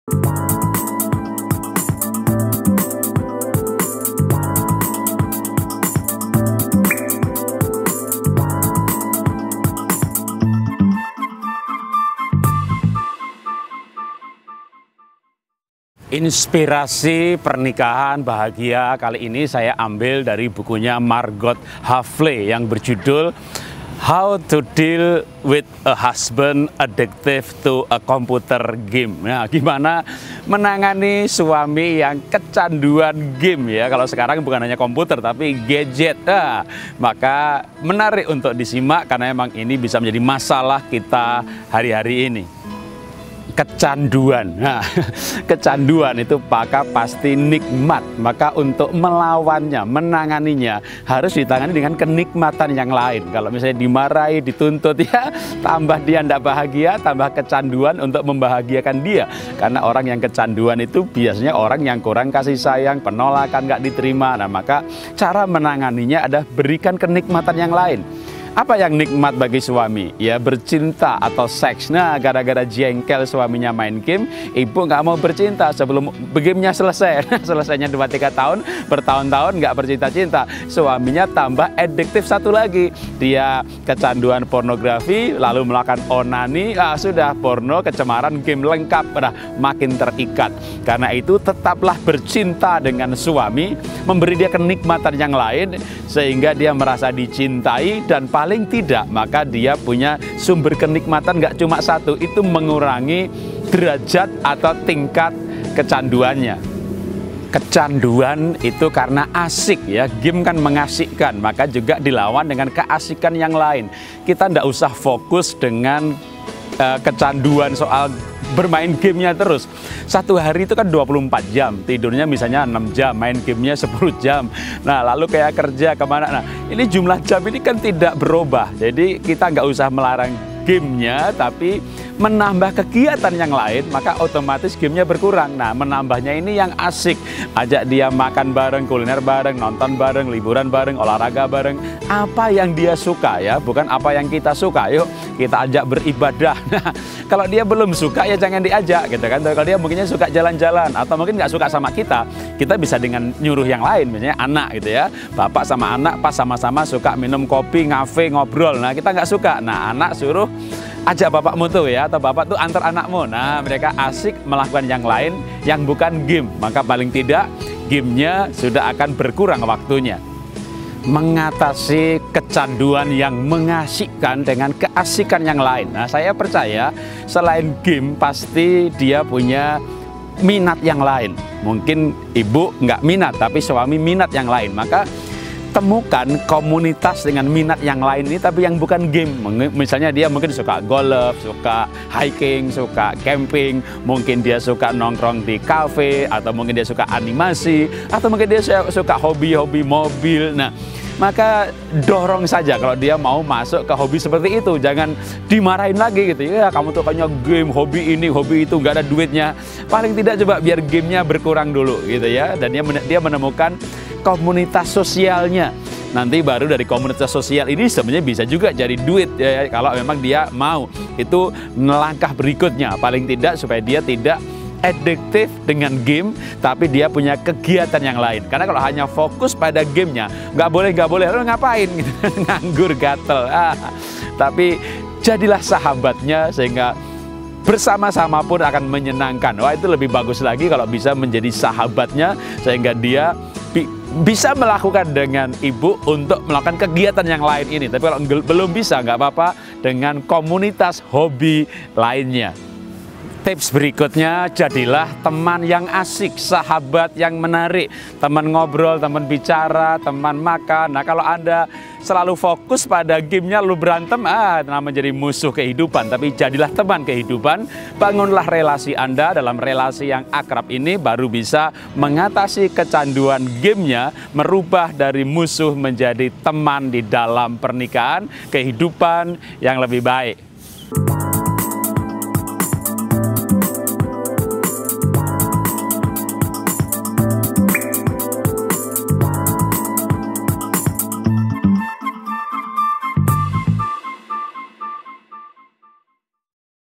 Suami Game Adiktif? Dr. Ir. Jarot Wijanarko M.Pd
Bukan anak tetapi suami yang kecanduan game? Kecanduan itu NIKMAT, maka ganti, alihkan, tawari
suami' beri keNIKMATan yang lain. Nah apa itu? Simak episode iniSuami Game Adiktif?  Dr. Ir. Jarot Wijanarko M.Pd